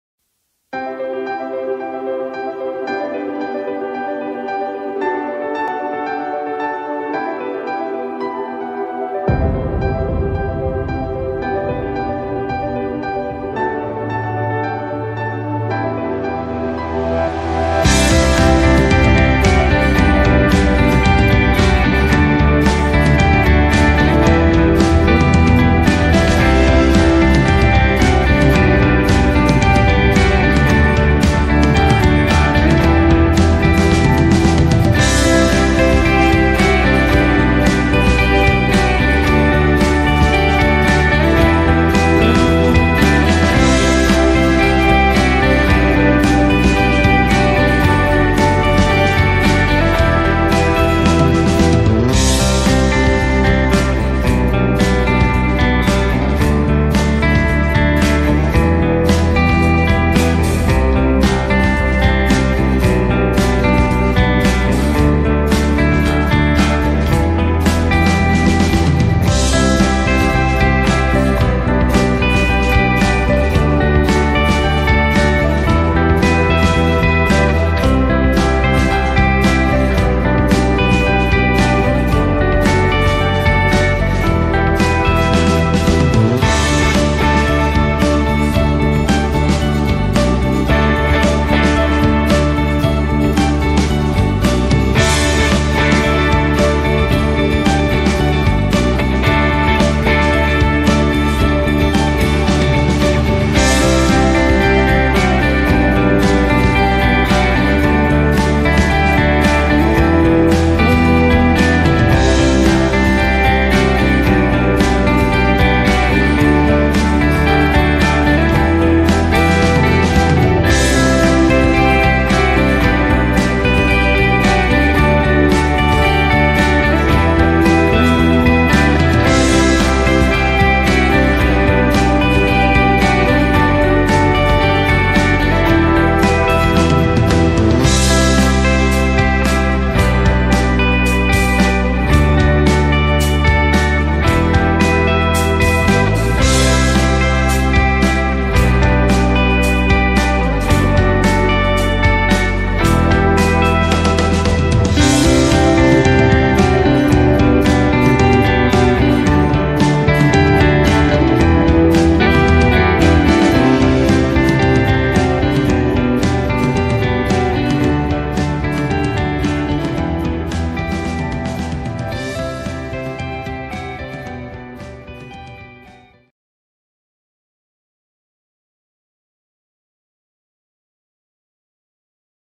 FLY DRONE MJX X102H + YICAM pangkalan susu
masih belajar nerbangin drone mjx + xiaomi yi